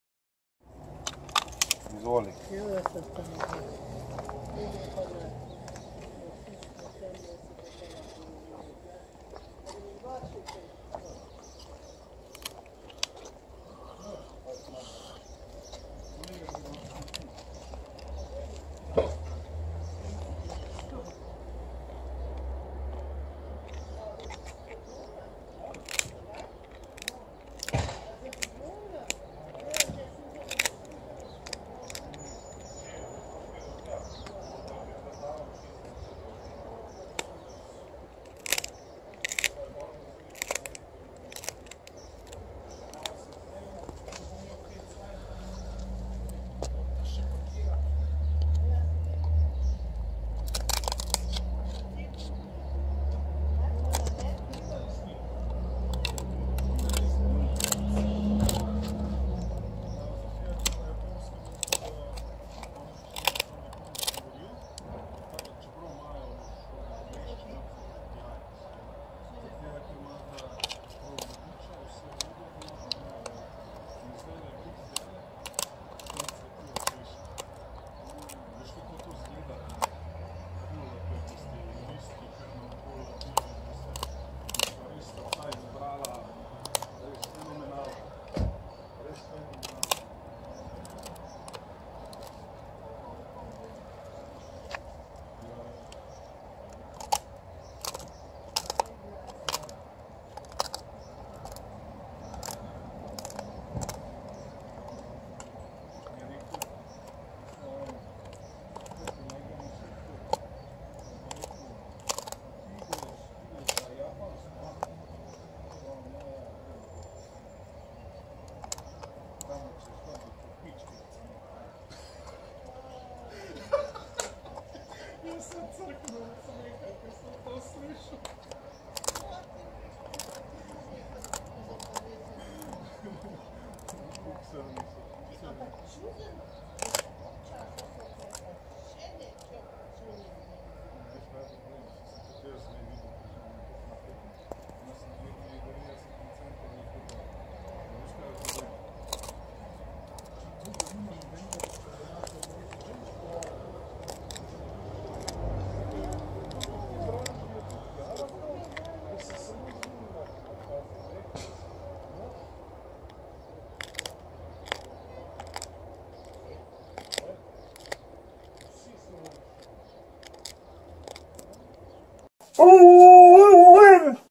Alaskan Malamute gets a crunchy treat and enjoys it outside | ASMR Doggy Style
Once again her favourite cousin came and as always she got a treat, which she finished it off outside, while also enjoying the sun.